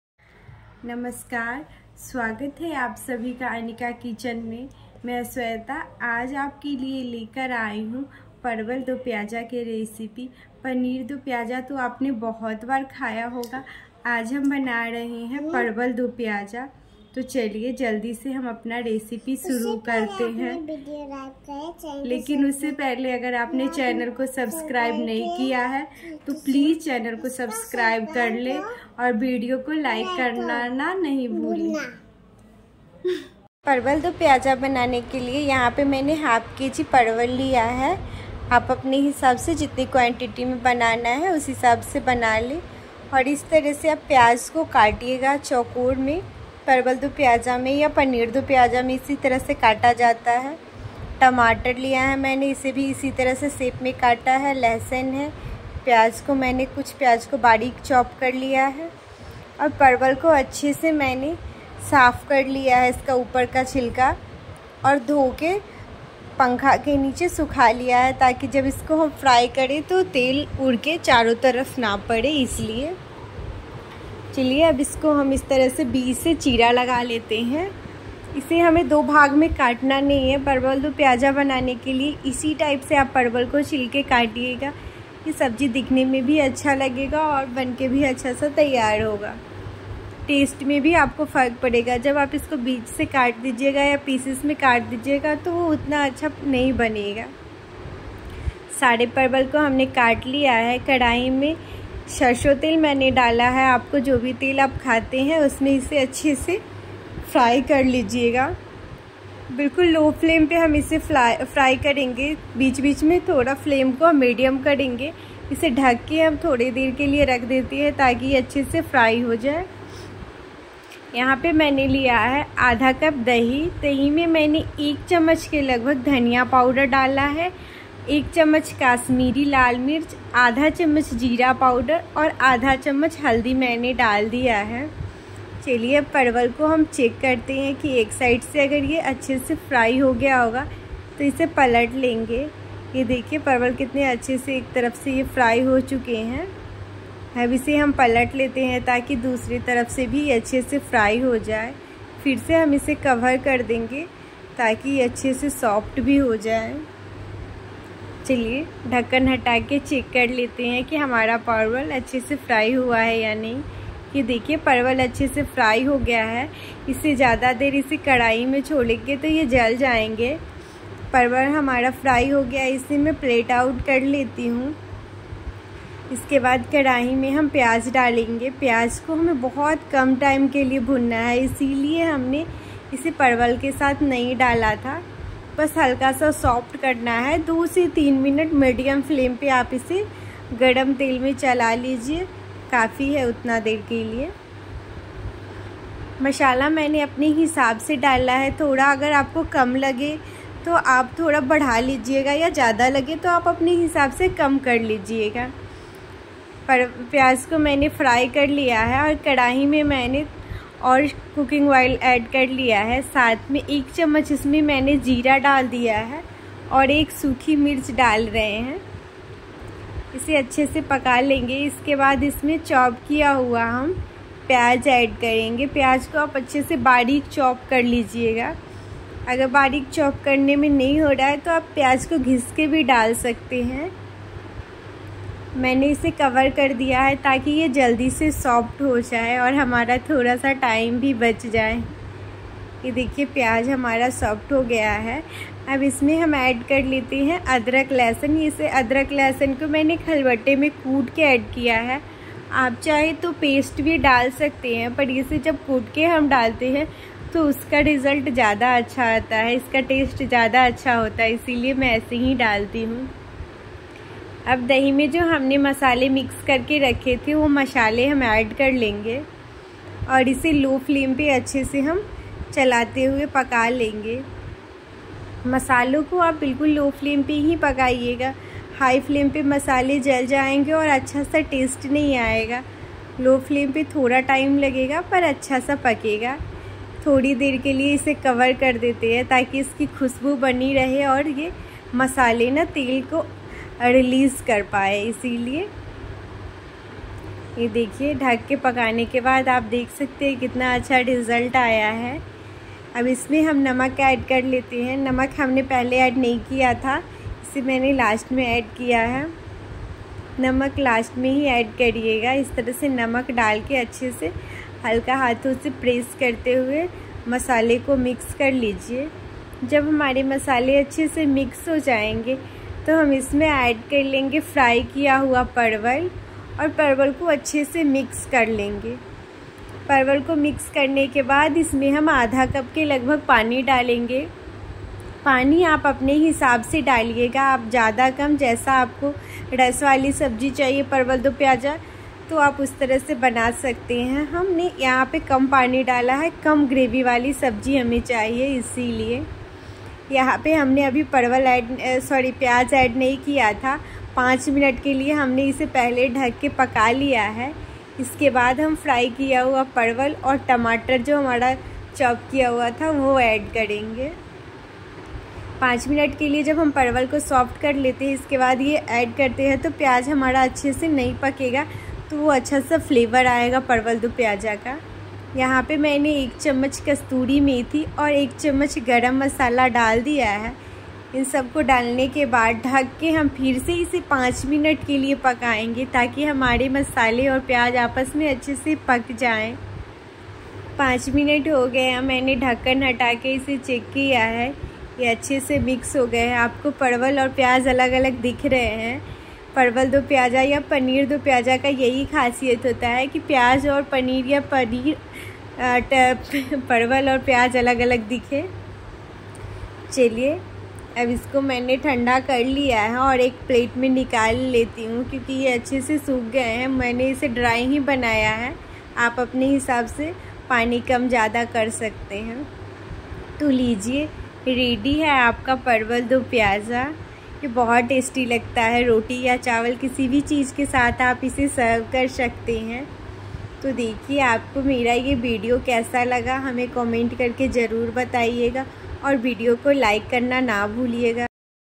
Parwal do pyaza ki recipe dhawa style se |parwal or pyaj ki sabji restaurant style |परवल दो प्याजा
🙏 thank for sporting
Mere sare YouTube family ko mera bahut sara pyar 😘 aap sblogo se request hai ki aap plz video ko like karna nhi bhule 👍🙏